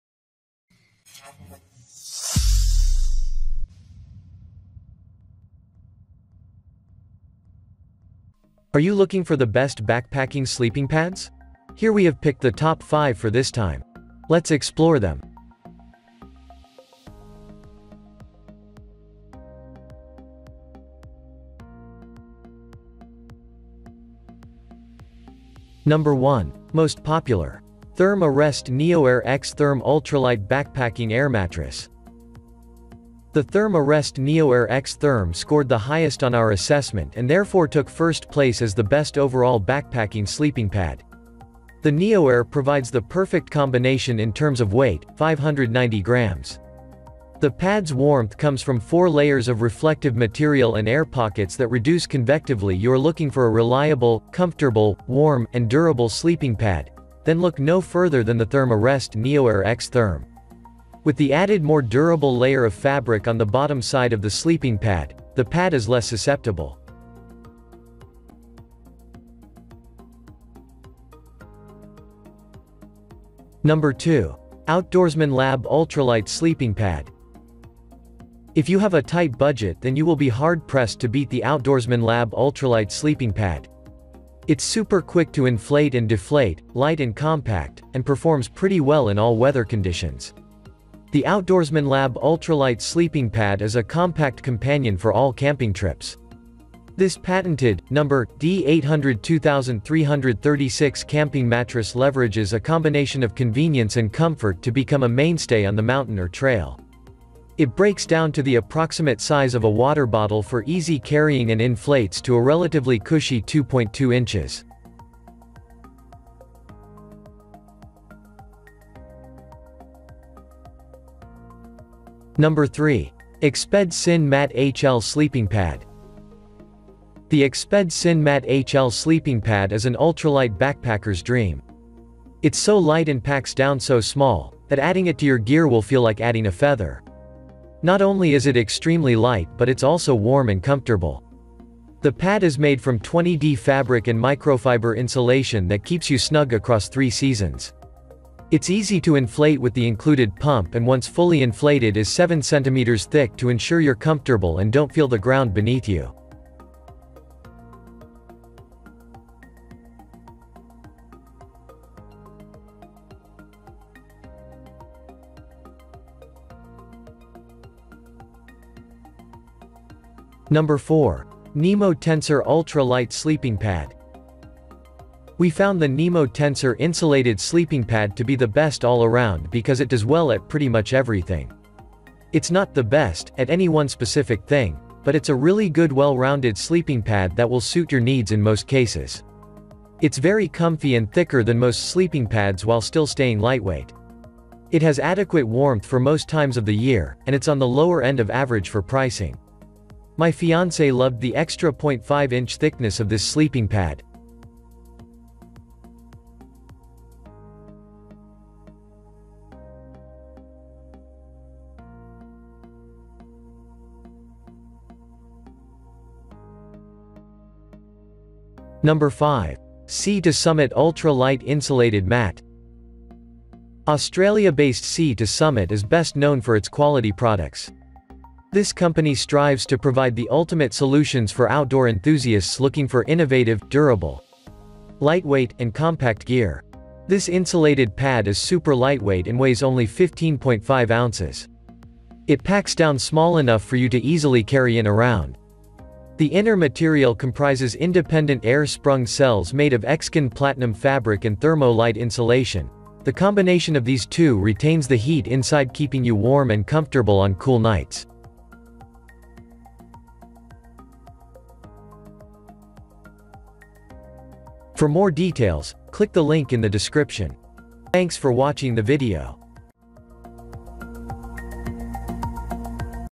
Top 5 Best Backpacking Sleeping Pads Review in 2022 | Make Selection From Our Recommended
Best Backpacking Sleeping Pads featured in this video:
0:25 1. Therm-a-Rest Ultralight Backpacking Air Mattress
1:14 2. Outdoorsman Lab - Ultralight Sleeping Pad
2:02 3. Exped SynMat HL Sleeping Pad
2:51 4. Nemo Tensor Ultralight Sleeping Pad
3:39 5. Sea to Summit Ultralight Insulated Mat

What is a backpacking sleeping pad?
Basically, a sleeping pad is something that a backpacker or camper can sleep on at night to be more comfortable. These pads are lightweight, and are designed to be carried on either weekend backpacking trips or longer expeditions. Thus, they differ from pads that are used for car camping, which are much heavier, or require an air pump to inflate.